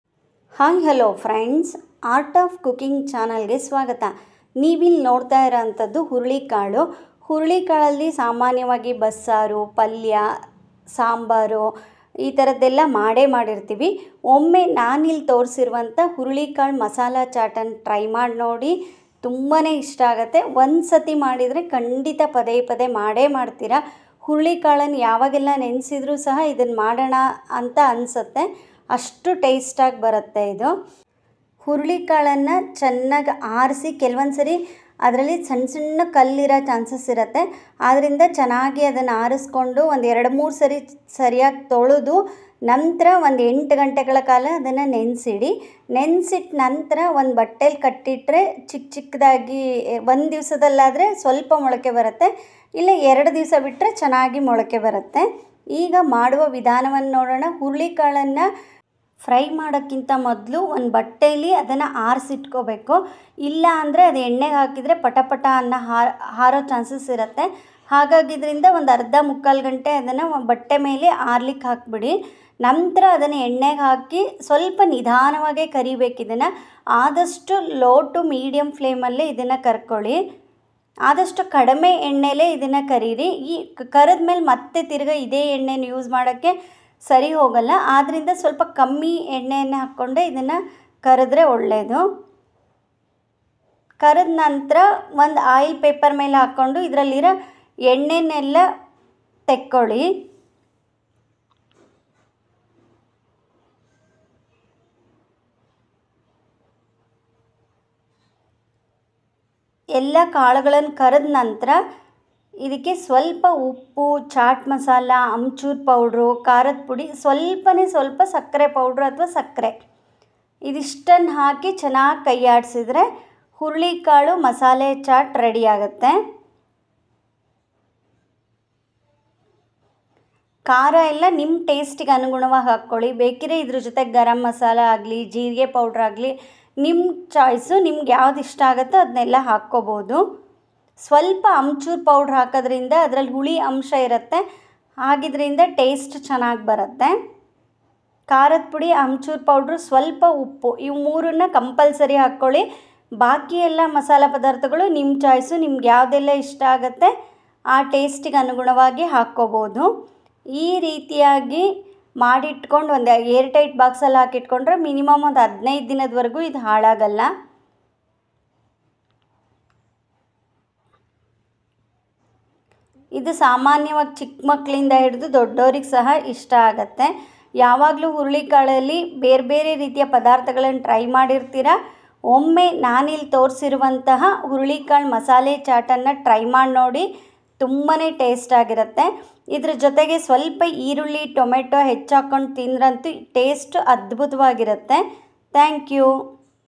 horse gram masala chat / ಮೊಳಕೆ ಹುರುಳಿಕಾಳಿನ ಮಸಾಲ ಚಾಟ್
Welcome to art of cooking channel, today recipe horse gram masala chaat, with detail photo and video,
 Ingredients
Horse gram,salt, oil, chat masala, red chilli powder, amchur powder Garam Masala, sugar.horse gram masala chat / ಮೊಳಕೆ ಹುರುಳಿಕಾಳಿನ ಮಸಾಲ ಚಾಟ್